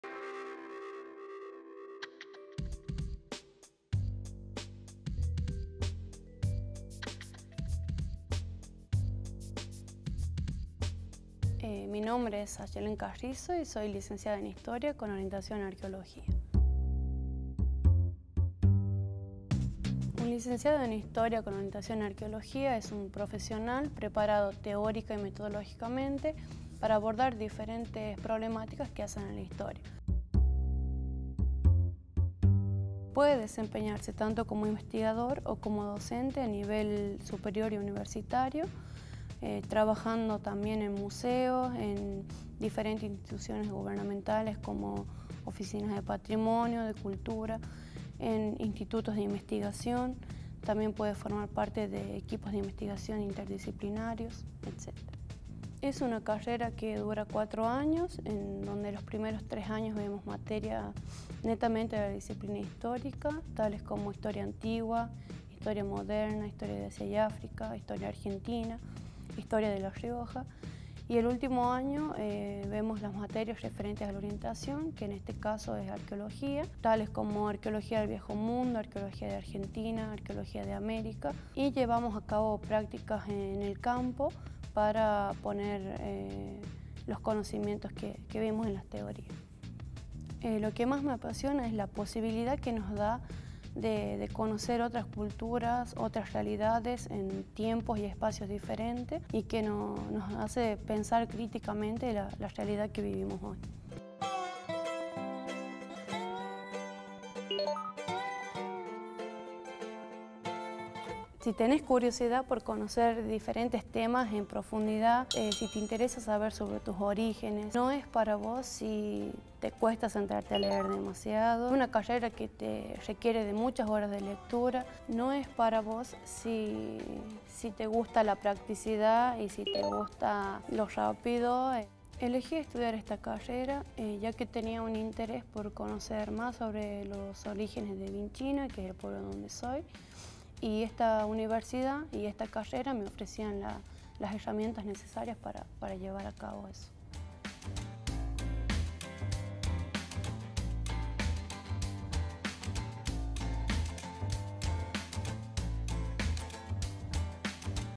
¿Te gustaría estudiar Historia en la UNLaR?
Jóvenes graduadas de la Licenciatura en Historia cuentan qué hace un profesional en su carrera, en qué y dónde puede trabajar y su experiencia personal en la vida universitaria.
"En Carrera" busca despertar y orientar vocaciones profesionales en futuros estudiantes que elijan la Universidad Nacional de La Rioja. Una coproducción con la Red Nacional Audiovisual Universitaria.